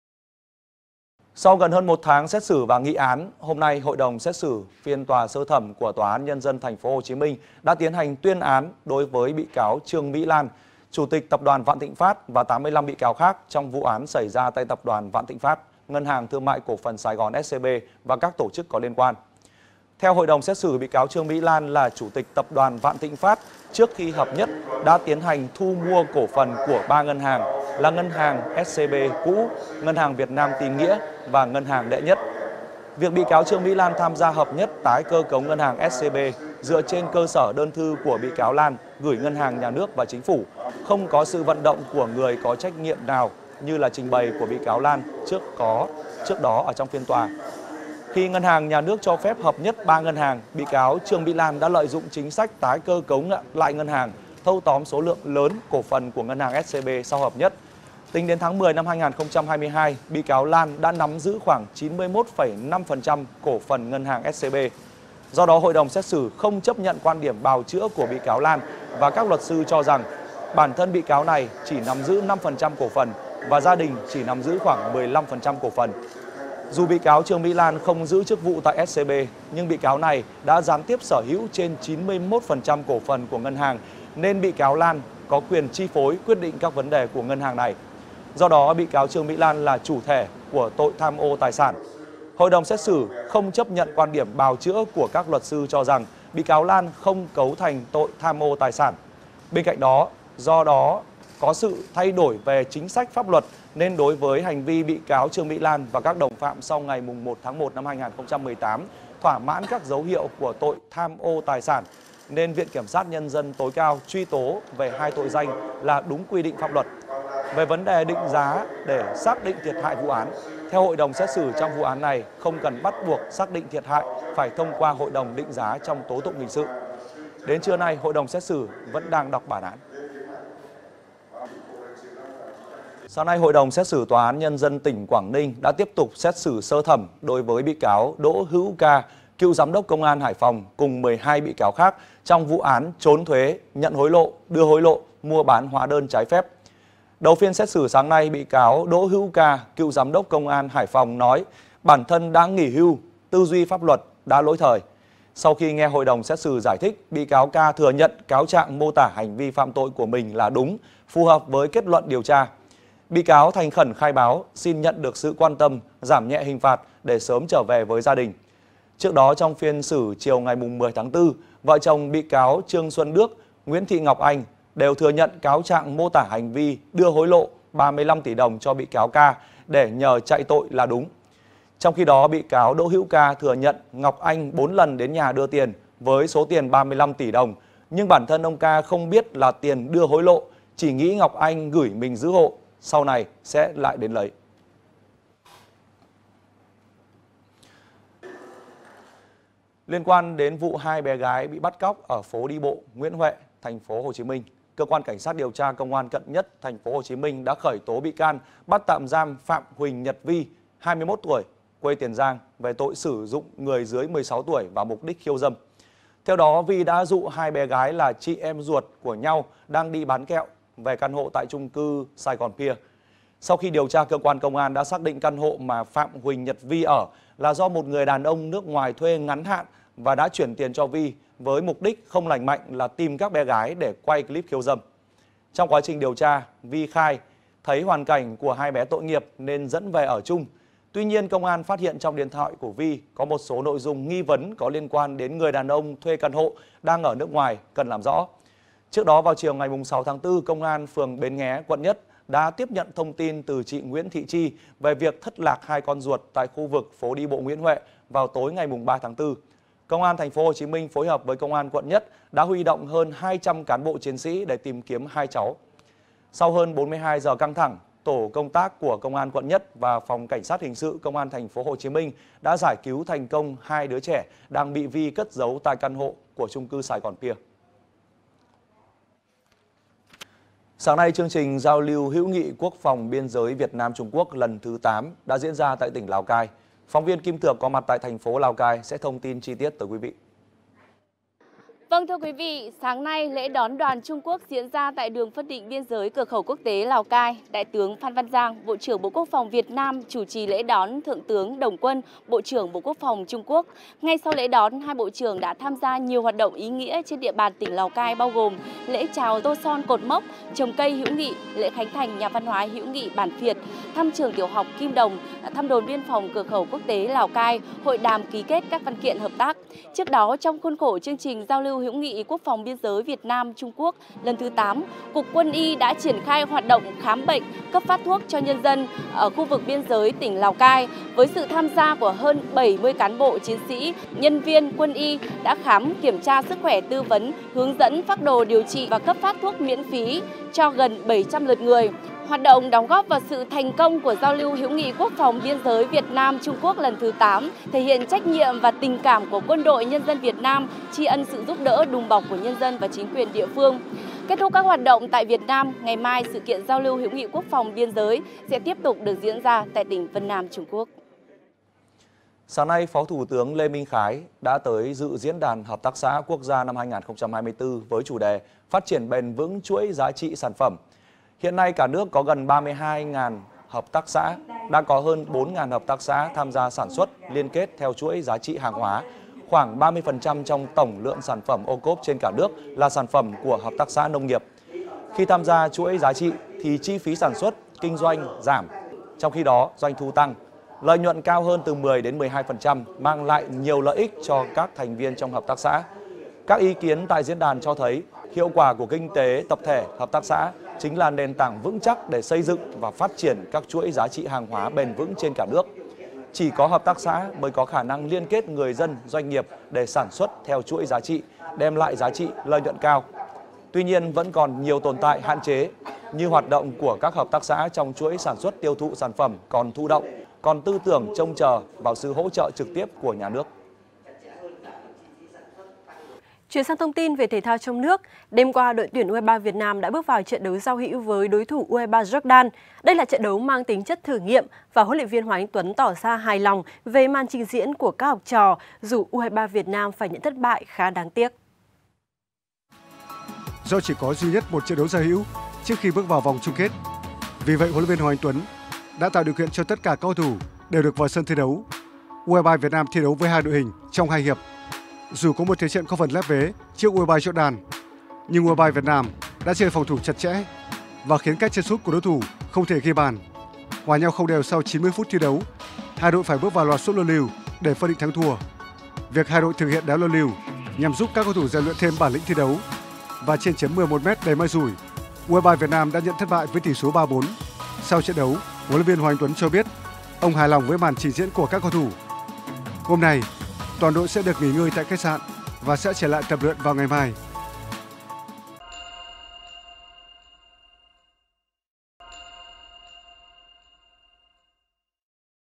Vụ Vạn Thịnh Phát: Bị cáo Trương Mỹ Lan gián tiếp sở hữu 91,5 % cổ phần SCB | VTC Now
VTC Now | Ngày 11/4, Tòa án nhân dân TPHCM tuyên án đối với bà Trương Mỹ Lan (Chủ tịch HĐQT Tập đoàn Vạn Thịnh Phát) và 85 bị cáo khác sau một tuần nghị án. Hội đồng xét xử không chấp nhận lời bào chữa của bị cáo Trương Mỹ Lan và các luật sư về việc bị cáo chỉ có 5% cổ phần,  và gia đình chỉ nắm giữ khoảng 15% cổ phần tại SCB. Hội đồng xét xử cho rằng là người gián tiếp sở hữu hơn 91,5% cổ phần nên bà Trương Mỹ Lan đã chi phối, điều hành toàn bộ hoạt động tại SCB.

(*) Tải ứng dụng VTC Now hoặc truy cập trang để không bỏ sót thông tin mới!